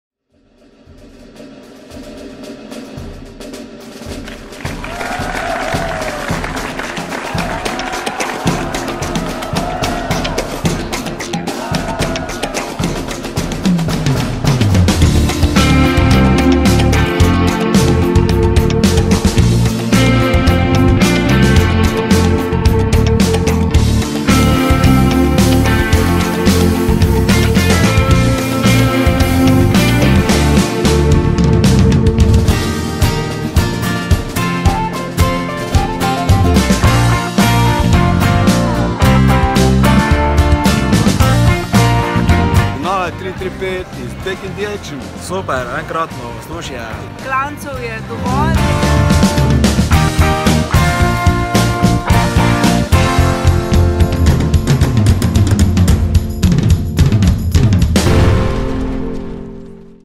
Diagonala 335 2018 Teaser Trailer 1 min
Od Goričkega do Portoroža na dolžini 335 km v dobrodelnem duhu

Trailer, Teaser 1 min